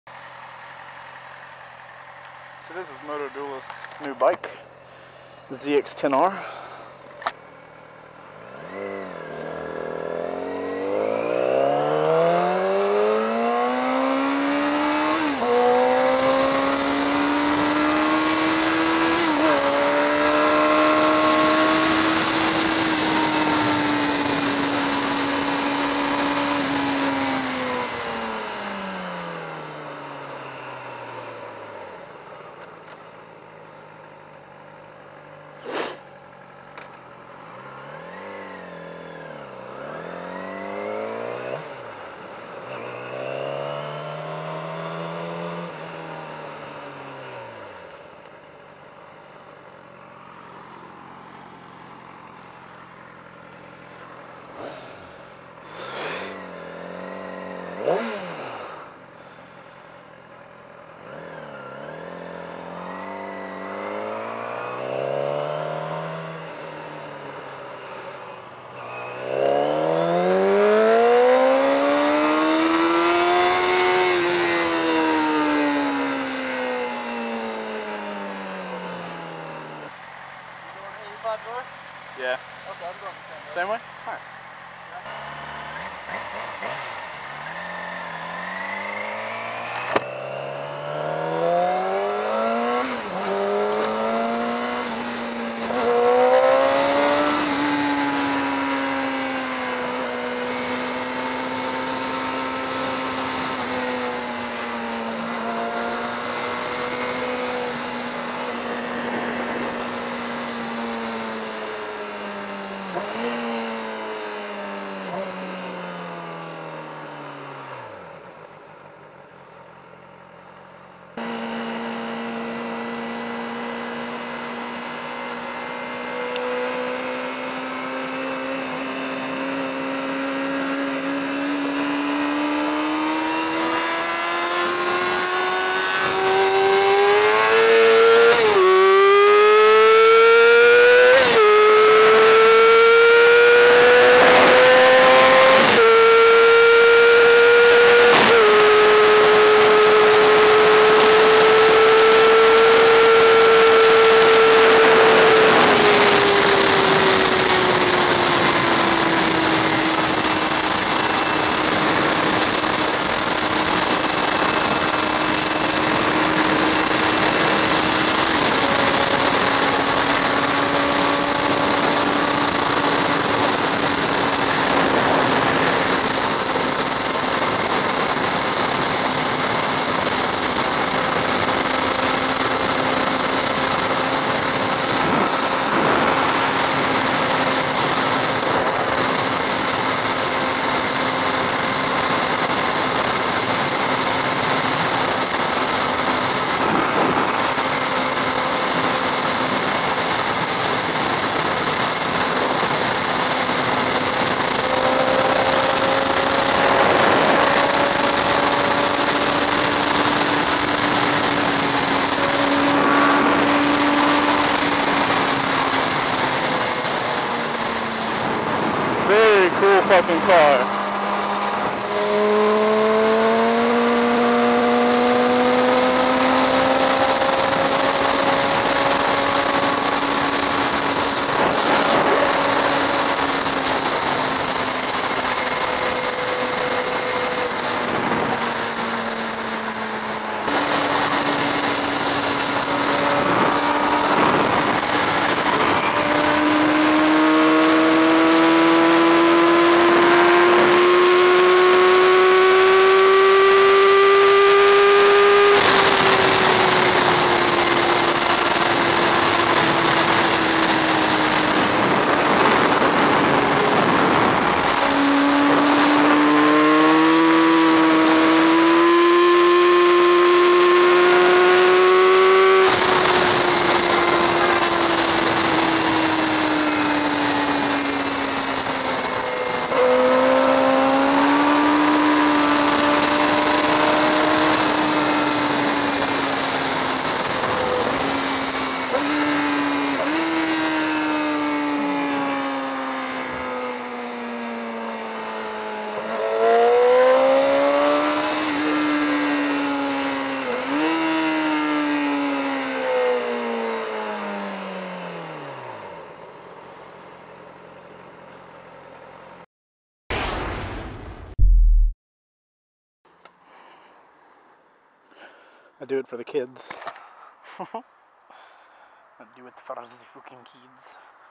ZX-10 and R6 play on the highway
A blast from the past for the people who miss the R6. See? A Kawasaki ZX10r and a Yamaha YZF-R6 *can* be friends!